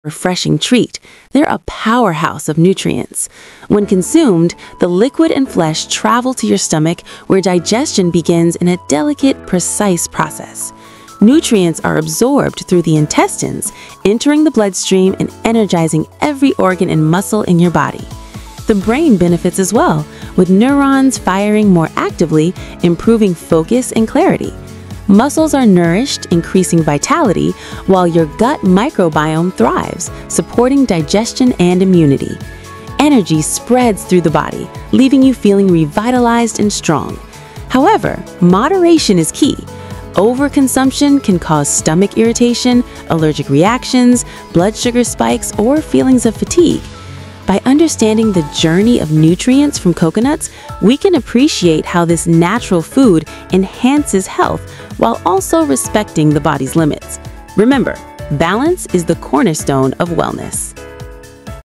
“Coconut Power: 3D Medical Visualization of Nutrients & Health Effects | Dr. Lily”
Discover the incredible journey of nutrients from coconuts through the human body in stunning 3D medical visualization. Dr. Lily explains how coconuts support digestion, brain function, muscle health, gut microbiome, and overall energy, while also highlighting potential side effects like stomach irritation, allergies, blood sugar spikes, and fatigue. Experience cinematic, realistic anatomical visuals that make science and nutrition come alive. Perfect for health enthusiasts, medical students, and anyone curious about the inner workings of the human body. Watch, learn, and see how balance in nutrition can transform your wellness.